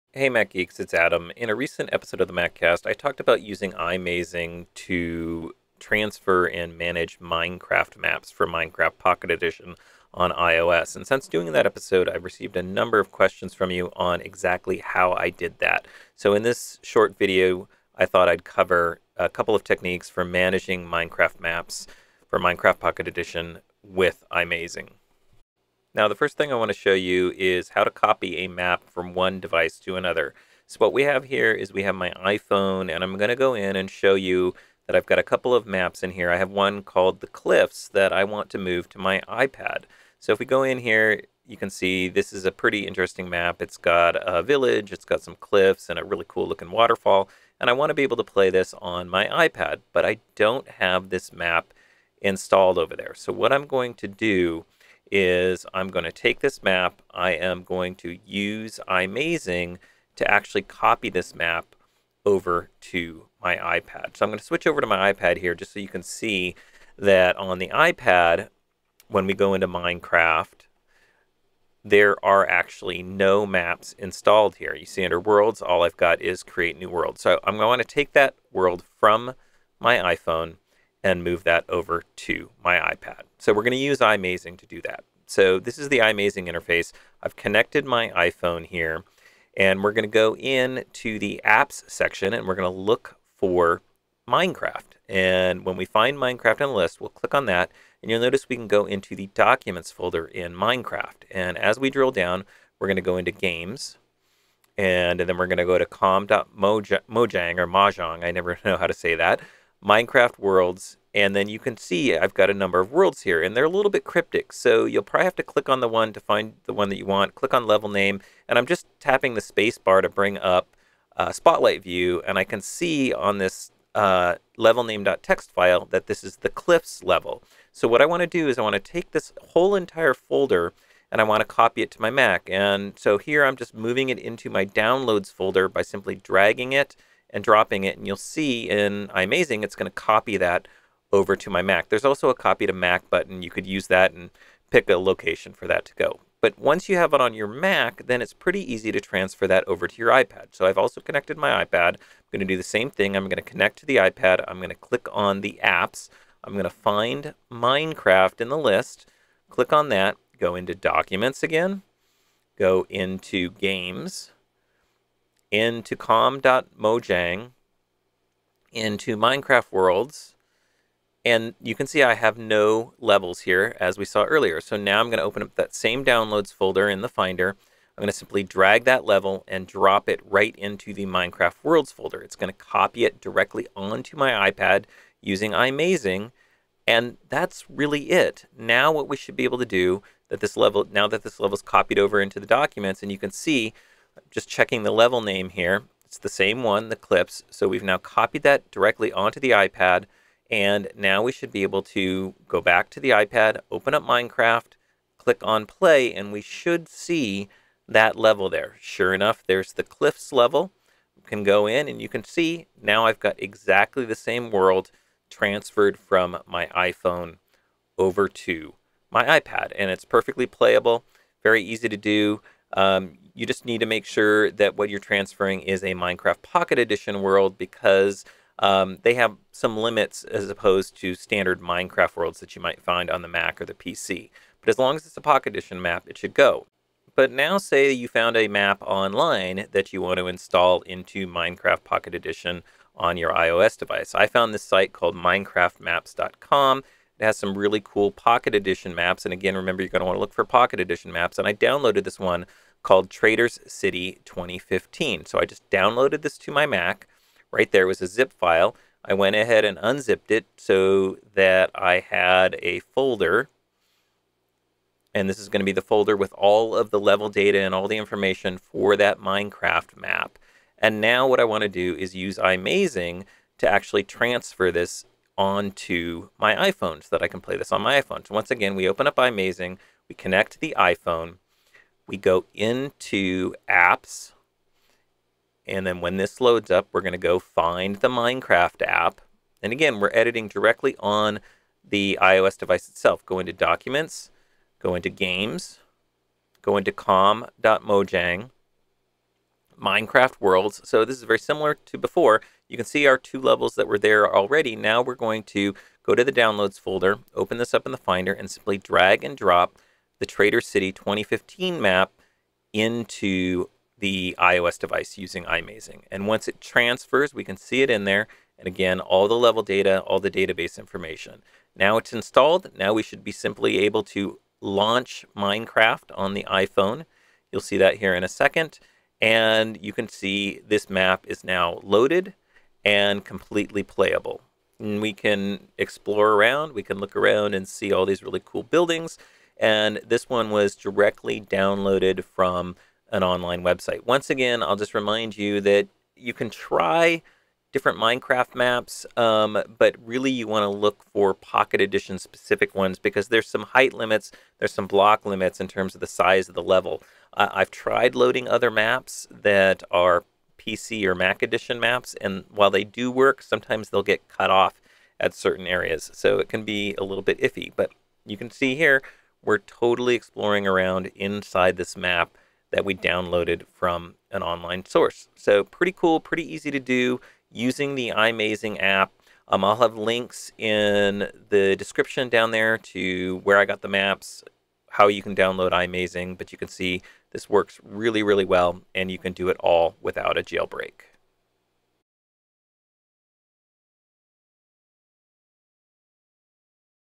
Maccast Minecraft iMazing Tutorial
On a recent Maccast episode I mentioned that I often will use the iMazing, app to manage and transfer Minecraft Pocket Edition maps onto or between my iOS devices without a jailbreak. After I made those comments many of you emailed to ask exactly how you do that. This is a quick how-to showing how I transfer levels between and iPhone and an iPad using iMazing. It also shows how to install a map download directly onto an iOS device using iMazing.